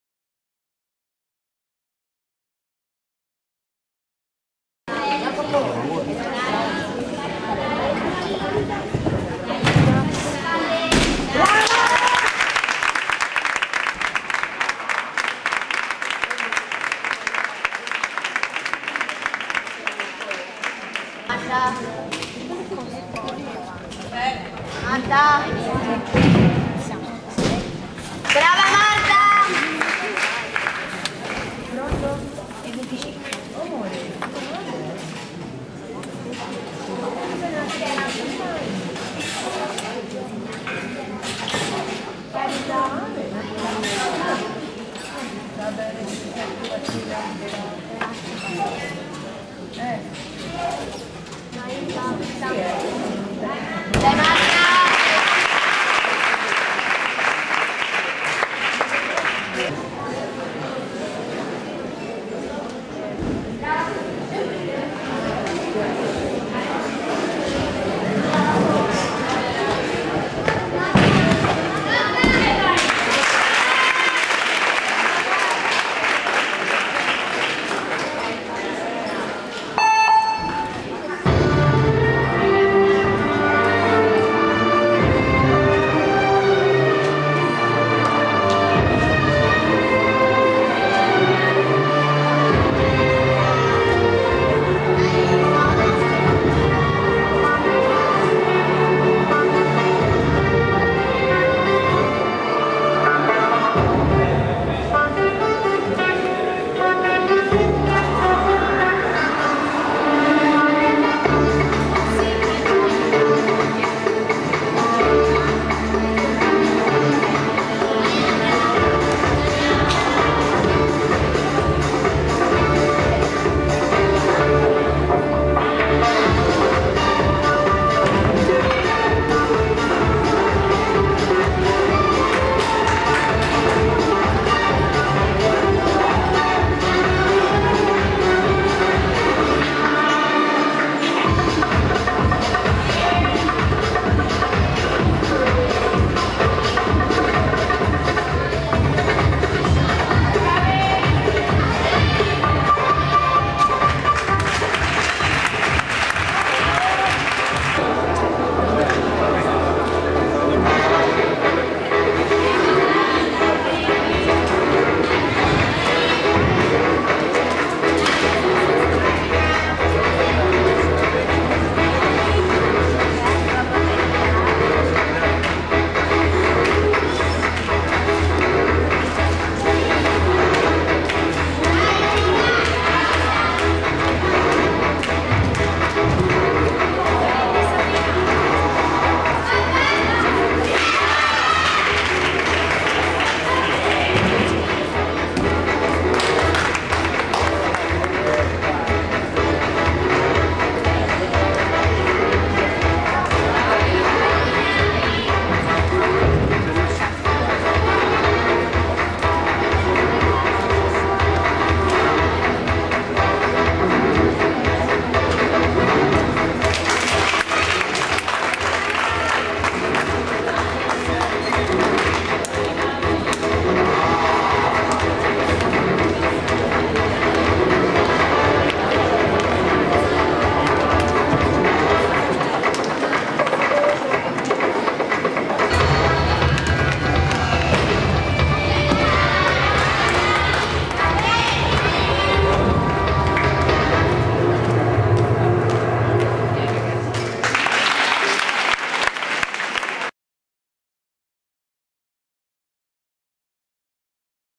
Lisa Isdraele Romano a Fano 2010
La gara di Lisa Isdraele Romano ( A.D. Polisportiva Casellina) ai Campionati Nazionali UISP di 6ª categoria Senior.